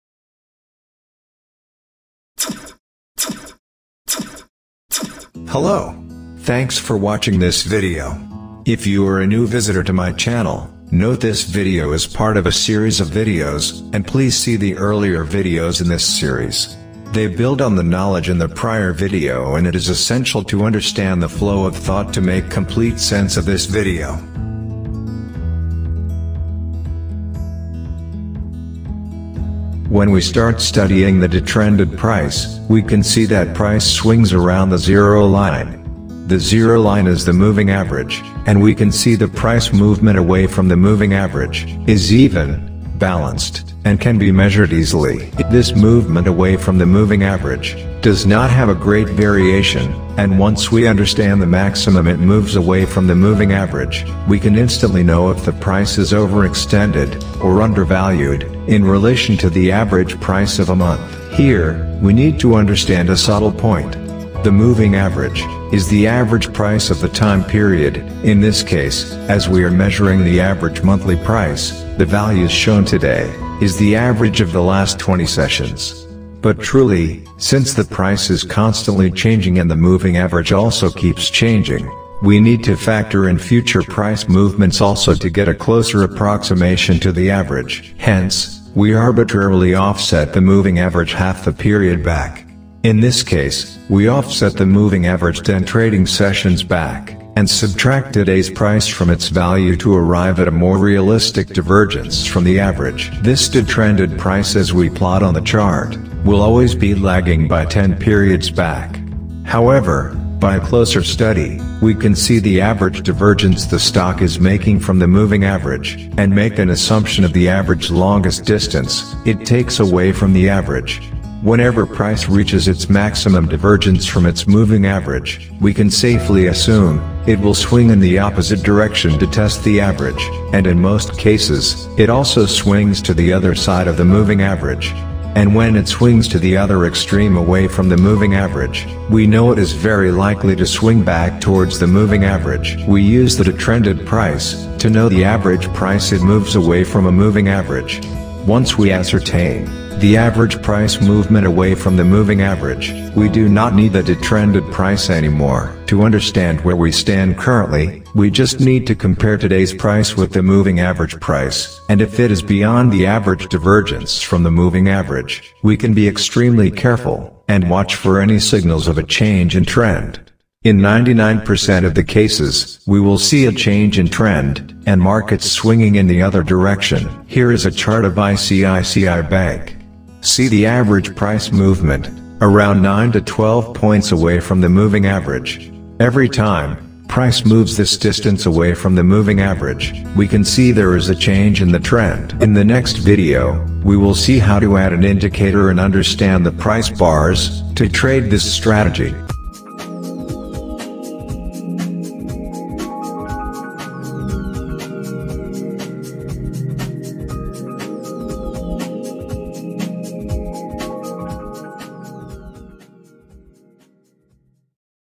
Trading Alpha to Omega 7 - A complete and comprehensive course on Technical Analysis
A Complete animated and comprehensive education of Technical Analysis for Financial Markets. Technical analysis looks for repeating price patterns related to persistent changes in investor sentiment and psychology.
Detrending Price. What is Detrending? and Why do we need to detrend the market action?